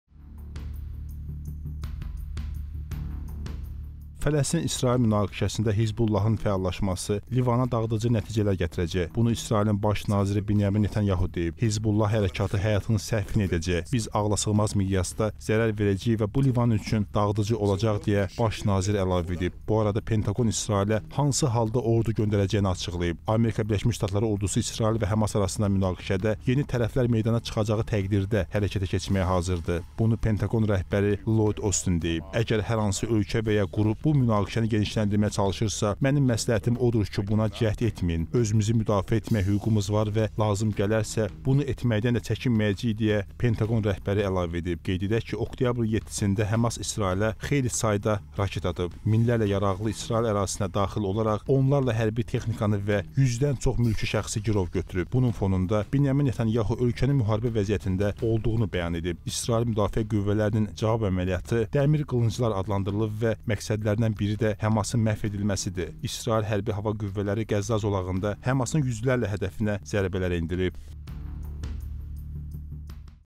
Netanyahudan Livana xəbərdarlıq: Biz ağlasığmaz miqyasda zərər verəcəyik
“Fələstin-İsrail münaqişəsində “Hizbullah”ın fəallaşması Livana dağıdıcı nəticələr gətirəcək”.
Bunu İsrailin Baş naziri Benyamin Netanyahu deyib.
“Hizbullah” hərəkatı həyatının səhvini edəcək. Biz ağlasığmaz miqyasda zərər verəcəyik və bu, Livan üçün dağıdıcı olacaq”, – deyə Baş nazir əlavə edib.
Bu arada, Pentaqon İsrailə hansı halda ordu göndərəcəyini açıqlayıb.
“ABŞ Ordusu İsrail və HƏMAS arasında münaqişədə yeni tərəflər meydana çıxacağı təqdirdə hərəkətə keçməyə hazırdır”. Bunu Pentaqon rəhbəri Lloyd Ostin deyib.
“Əgər hər hansı ölkə və ya qrup bu münaqişəni genişləndirməyə çalışırsa, mənim məsləhətim odur ki, buna cəhd etməyin. Özümüzü müdafiə etmək hüququmuz var və lazım gələrsə, bunu etməkdən də çəkinməyəcəyik”, – deyə Pantaqon rəhbəri əlavə edib.